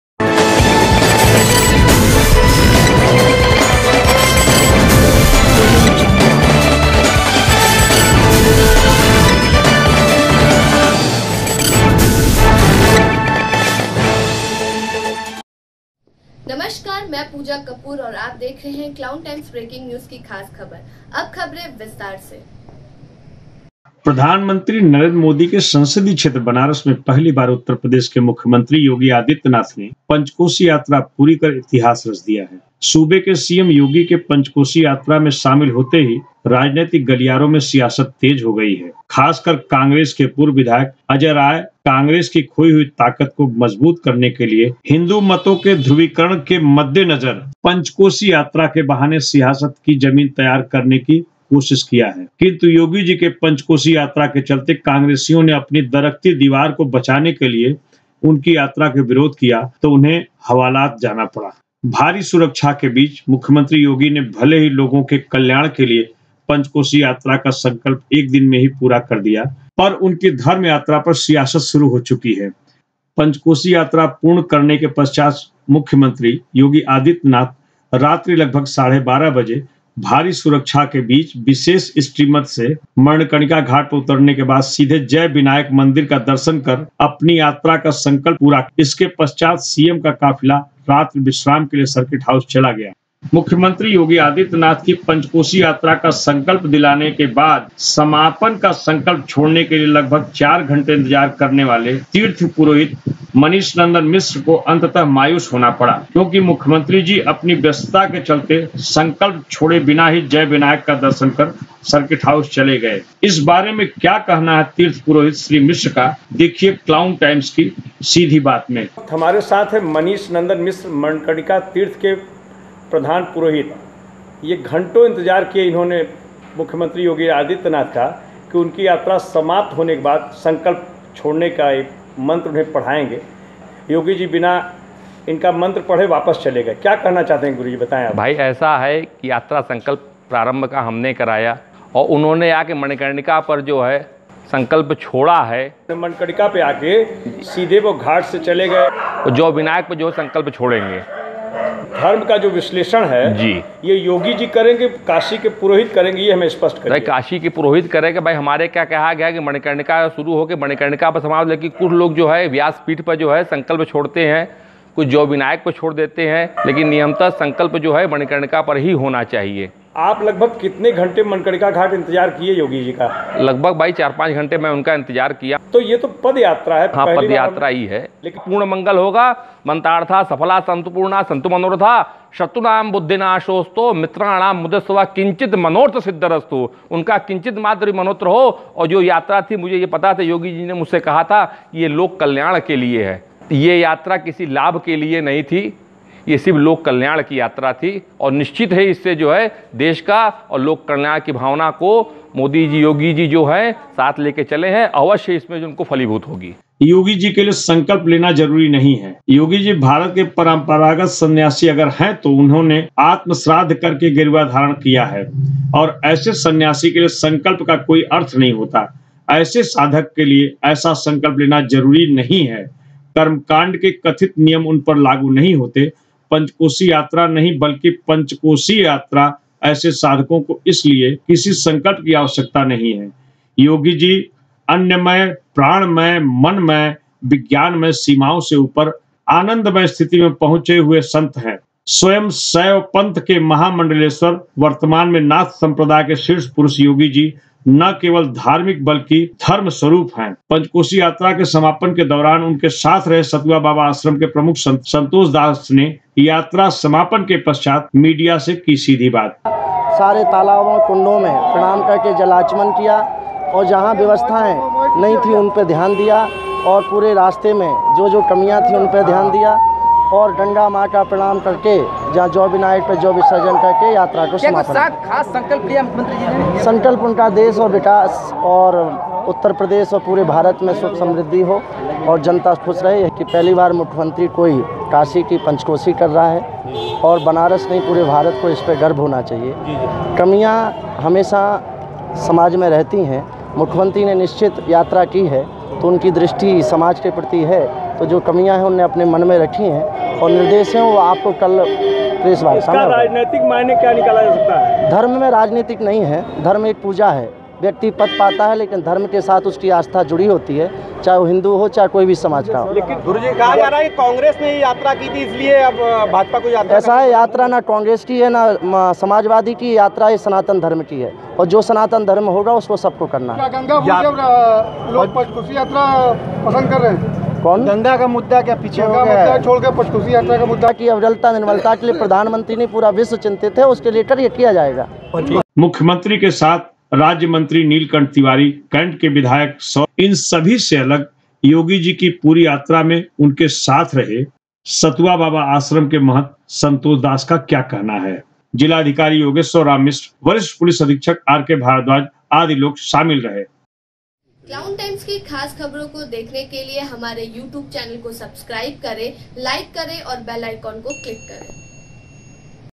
Clown Timesसीएम योगी ने पीएम मोदी के संसदीय क्षेत्र बनारस में पंचक्रोशी यात्रा पूर्ण कर रचा इतिहास
सूबे के सीएम योगी के पंचक्रोशी यात्रा में शामिल होते ही राजनीतिक गलियारों में सियासत तेज हो गई है, खासकर कांग्रेस के पूर्व विधायक अजय राय कांग्रेस की कोई ताकत को मजबूत करने के लिए हिंदू वोटों के ध्रुवीकरण के मद्देनजर पंचकोशी यात्रा के बहाने सियासत की जमीन तैयार करने की कोशिश किया, किंतु योगीजी के पंचक्रोशी यात्रा के चलते कांग्रेसियों ने अपनी दरकती दीवार को बचाने के लिए उनकी यात्रा का विरोध किया तो उन्हें हवालात जाना पड़ा। भारी सुरक्षा के बीच मुख्यमंत्री योगी ने भले ही लोक के कल्याण के लिए पंचकोशी यात्रा संकल्प एक दिन में पूरा कर लिया किंतु उनकी धर्म यात्रा पर सियासत शुरू हो चुकी है। इन सब से अलग योगीजी की पूरी यात्रा में उनके साथ रहे सतुआ बाबा आश्रम के महंत संतोष दास का क्या कहना है।
चक्रपुष्करणी कुण्ड पर संकल्प दिलाने वाले तीर्थ पुरोहित मनीष नंदन मिश्र का क्या कहना है।